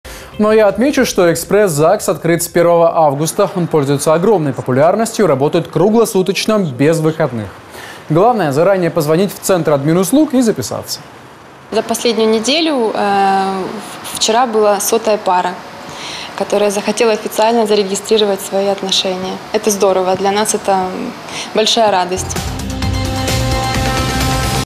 В экспресс-ЗАГСе зарегистрировали сто пар
Экспресс-загс на проспекте Шевченко работает с 1 августа. Он пользуется огромной популярностью и работает круглосуточно и без выходных. Главное, заранее позвонить в Центр админуслуг и записаться на роспись. Директор ГП «Центр обслуживания граждан» Виктория Литвин рассказала:

    «За последнюю неделю вчера была сотая пара, которая захотела официально зарегистрировать свои отношения. Это здорово. Для нас это большая радость».